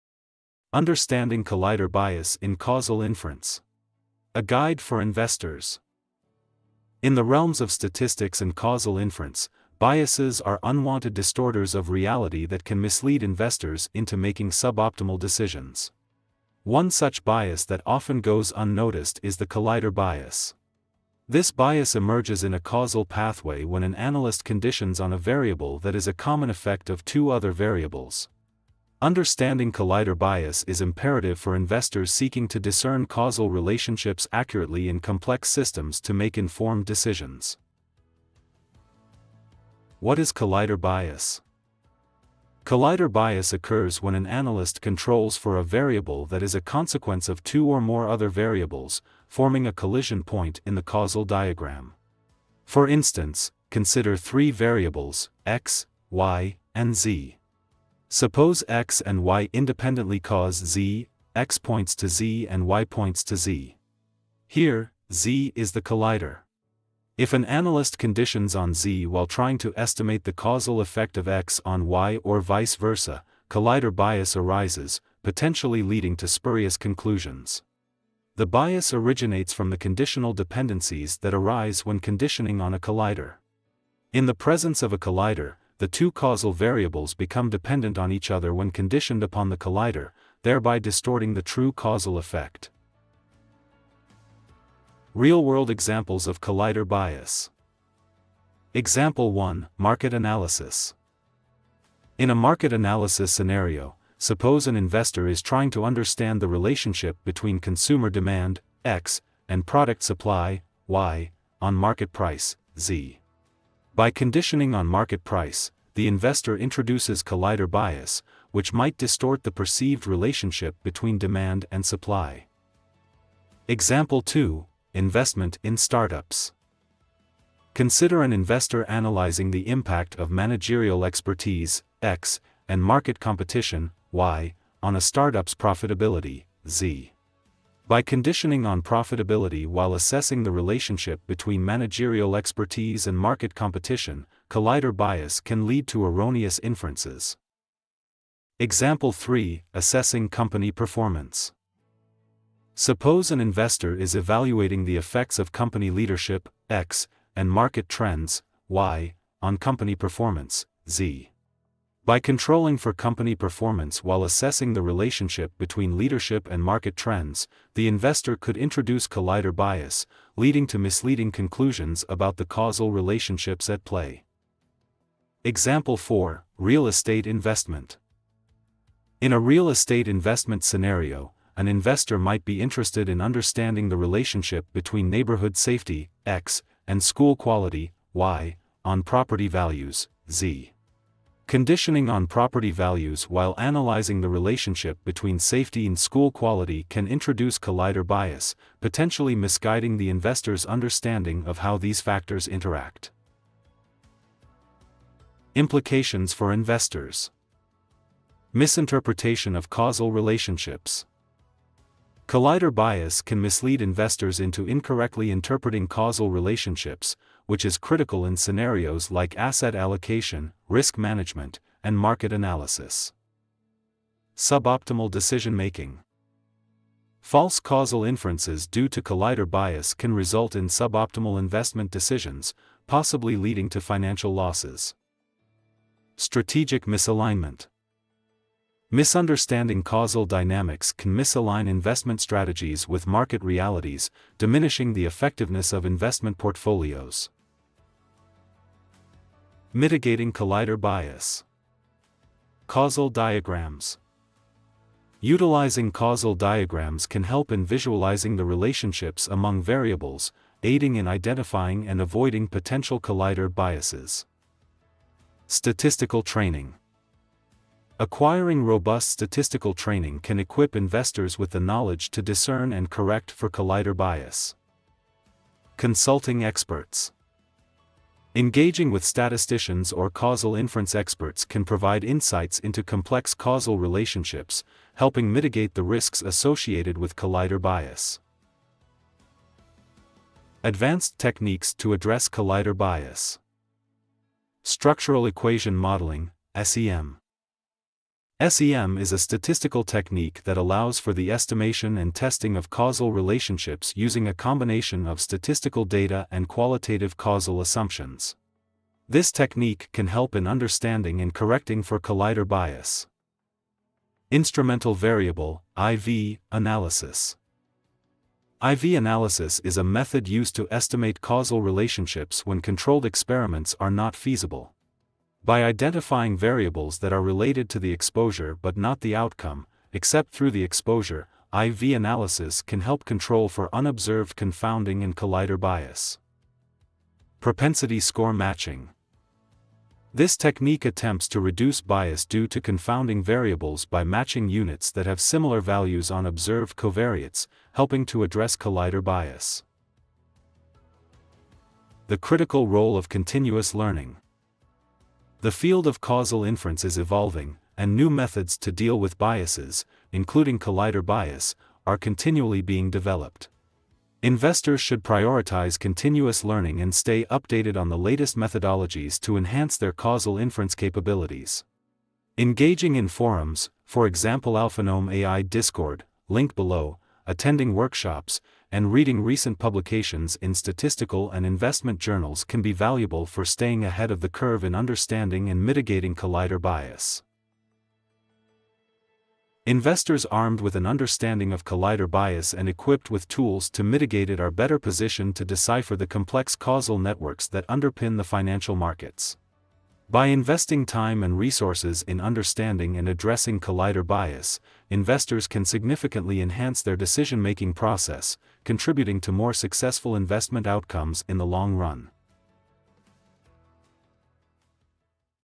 Understanding Collider Bias in Causal Inference: A Guide for Investors
In the realms of statistics and causal inference, biases are unwanted distorters of reality that can mislead investors into making suboptimal decisions. One such bias that often goes unnoticed is the collider bias. This bias emerges in a causal pathway when an analyst conditions on a variable that is a common effect of two other variables. Understanding collider bias is imperative for investors seeking to discern causal relationships accurately in complex systems to make informed decisions.